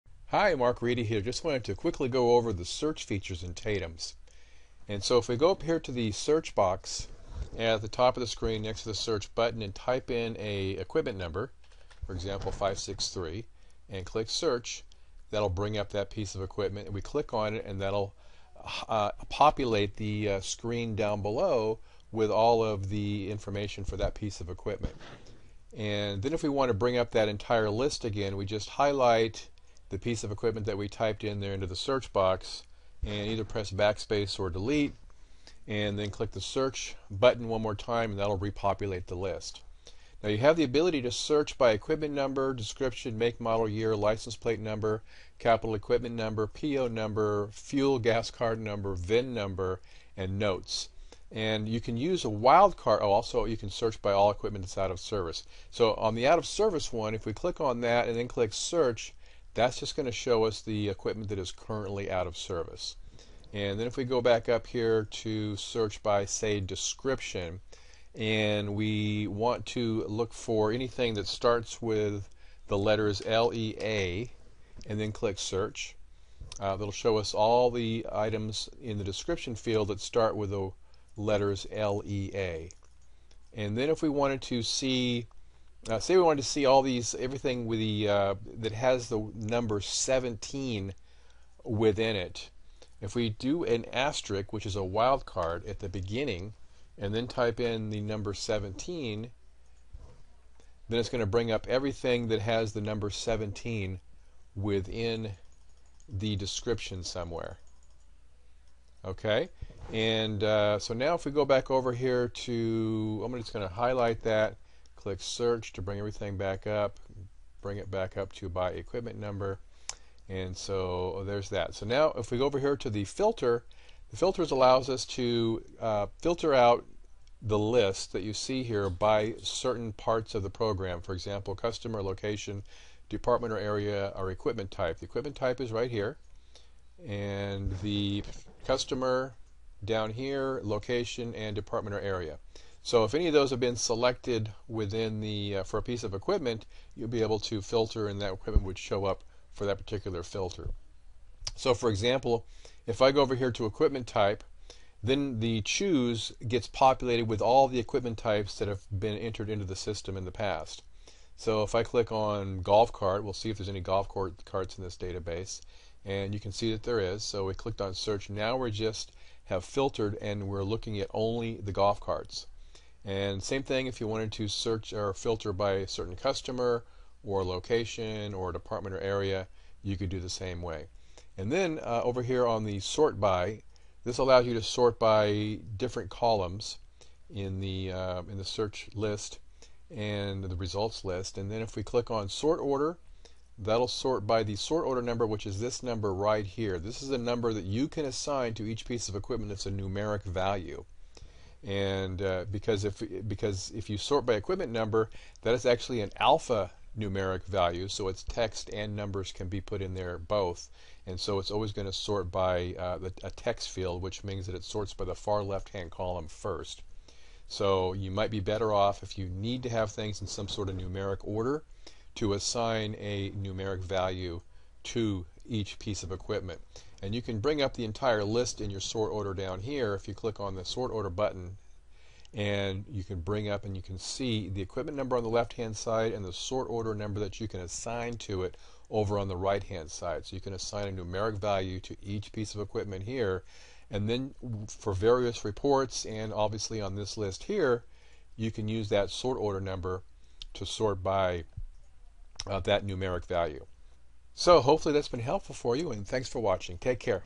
TATEMS Maintenance Software Search Function Help Video
Maintenance Software Search function Help video for TATEMS.

This video was created to provide a quick help overview showing you how to use the search function features on the TATEMS Maintenance Software Main Screen